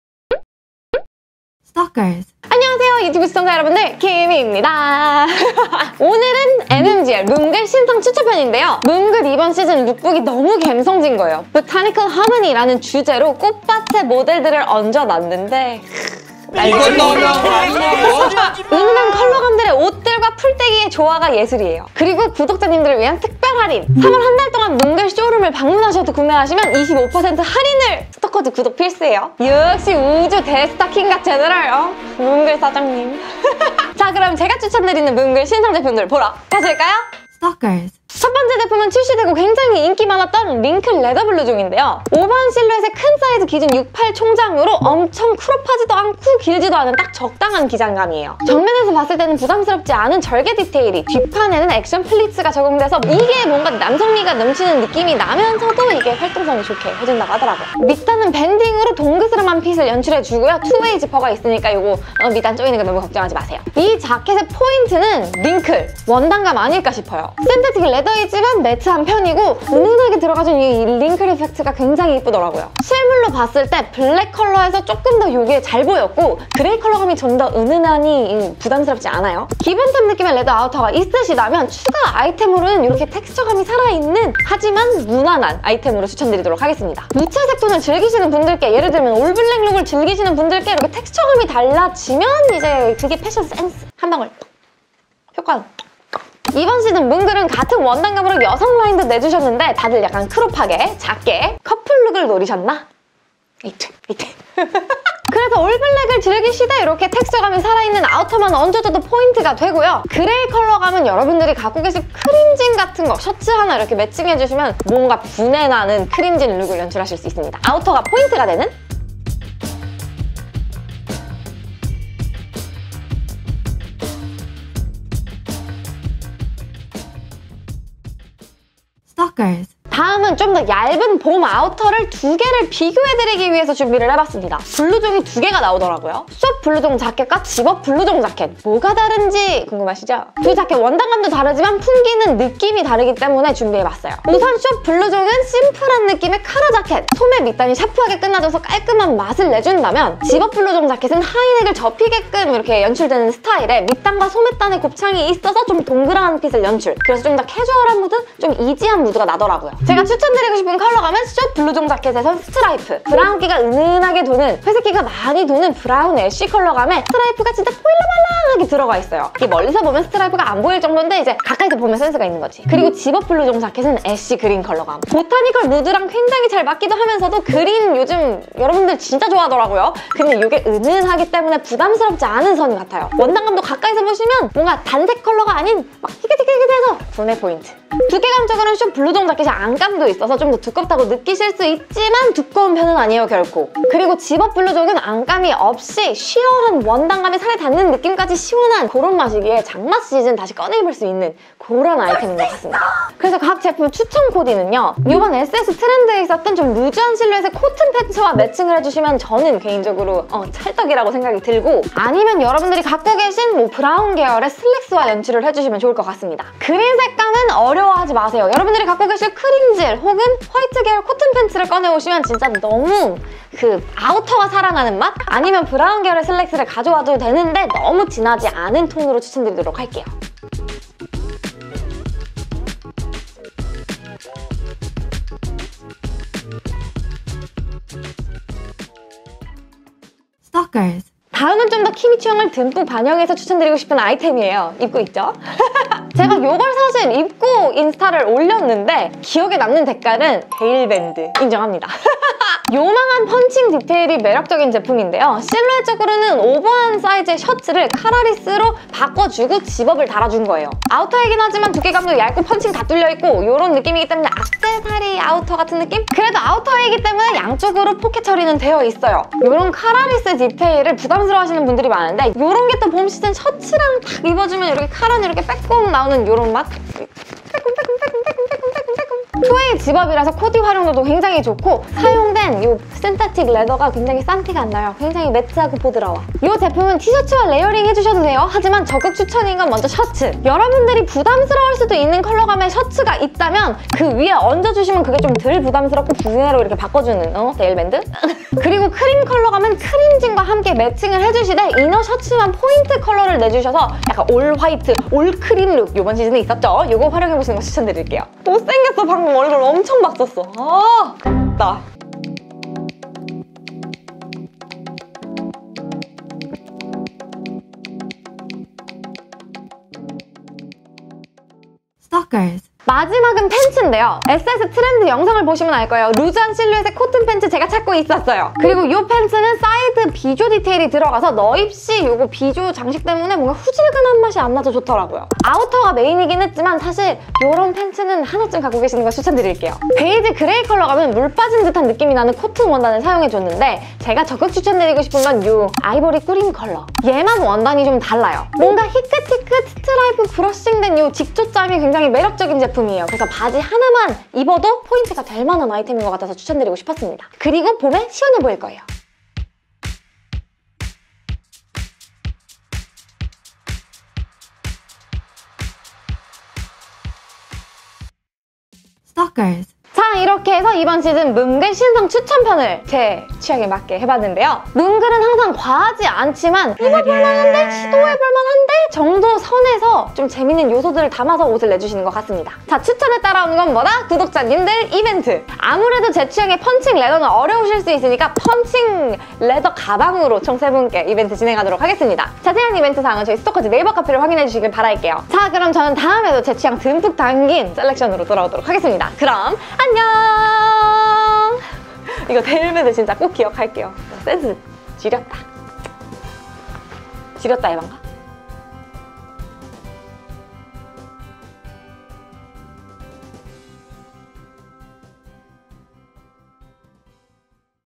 신상 할인은 못 참지
***오늘 MMGL의 금전 지원과 제품 지원을 받아서 만들어졌습니다***
**3월 한달동안 MMGL 쇼룸을 방문시
25% 할인 받으실 수 있습니다**

오늘은 MMGL 봄 신상 아우터들을 준비했습니다!
고급스럽고 우아한 무드의 신상 아우터들도 소개시켜드리고
MMGL 쇼룸을 방문하시면 최대 25% 할인까지 해주신다니
이번 기회에 할인 받으셔서 현명한 소비하셨으면 좋겠습니다ㅎㅎ

🌟PRODUCT INFO🌟 - 모델 스펙 183cm 70kg (매니정)

(1) WRINKLED LEATHER BLOUSON - 모델 4 사이즈 착용

(2) SHORT BLOUSON JACKET STRIPE  -모델 4 사이즈 착용

(3) ZIP-UP BLOUSON JACKET ASH GREEN -모델 4 사이즈 착용

(4) PUNCHED LEATHER JACKET -모델 4 사이즈 착용

(5) COTTON WIDE PANTS -모델 3 사이즈 착용

0:00 오프닝
0:37 WRINKLED LEATHER BLOUSON
1:58 WRINKLED LEATHER BLOUSON 매니정 코멘트
2:08 SHORT BLOUSON JACKET / ZIP UP BLOUSON JACKET
3:50 SHORT BLOUSON JACKET / ZIP UP BLOUSON JACKET 매니정 코멘트
4:00 PUNCHED LEATHER JACKET
5:11 PUNCHED LEATHER JACKET 매니정 코멘트
5:21 COTTON WIDE PANTS
5:58 COTTON WIDE PANTS 매니정 코멘트
6:08 엔딩